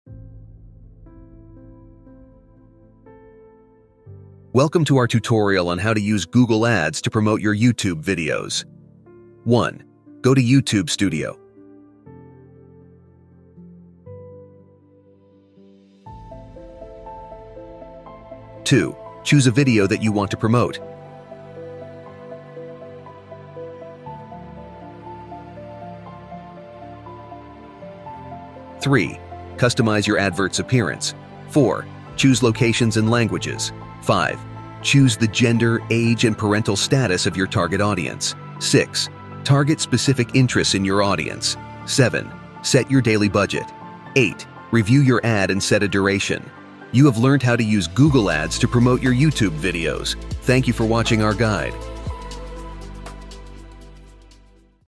How To Promote YouTube Videos On Google Ads
Are you looking to boost the visibility of your YouTube videos? Wondering how to reach a broader audience beyond YouTube's platform? Look no further! In this video, we'll delve into the art of promoting your YouTube videos using Google Ads. Whether you're a seasoned content creator or just starting your YouTube journey, understanding how to leverage Google Ads effectively can make a significant difference in your video's success.